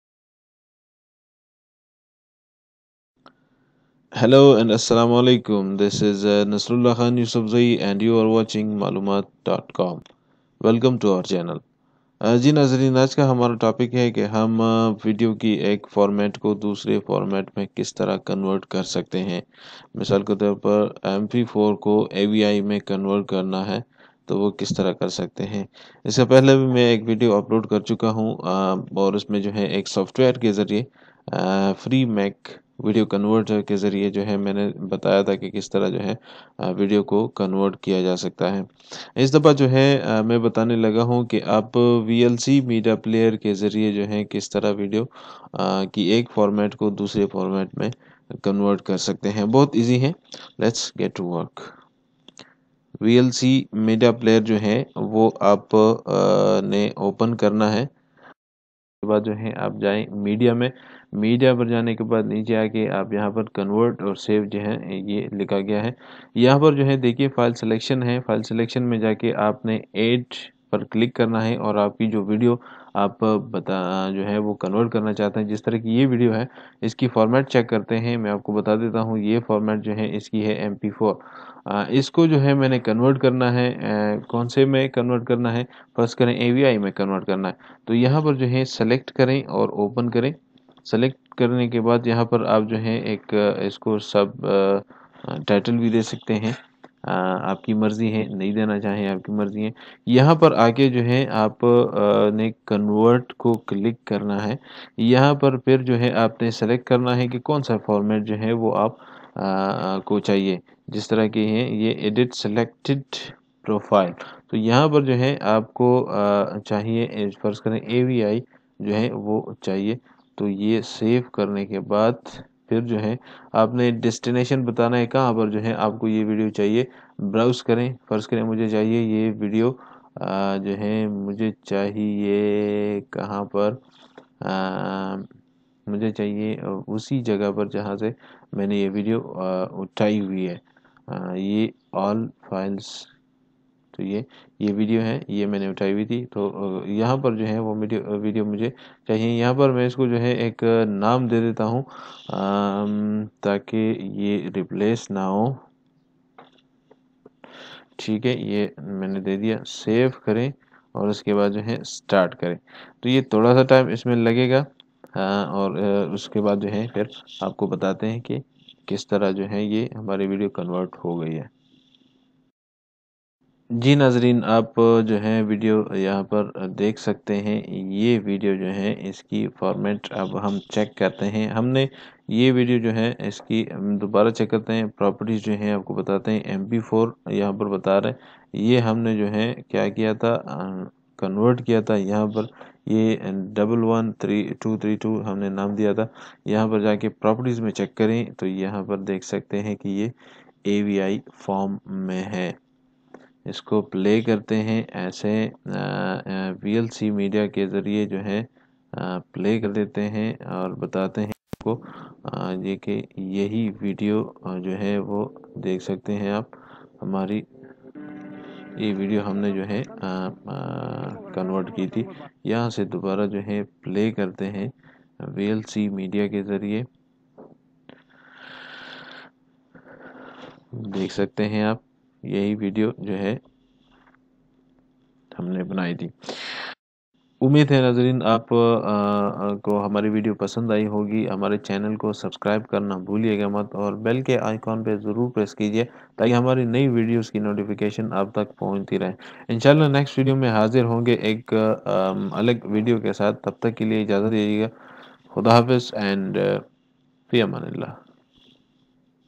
how to change video format using vlc media player, urdu/hindi full tutorial?
hello every one this is nasrullah khan yousafzai and you are watching MALOOMAT.COM
I am presenting a new video regarding how to change video format using vlc media player.
you can change any format as avi,mp4, mpeg also to mp3 using vlc media player.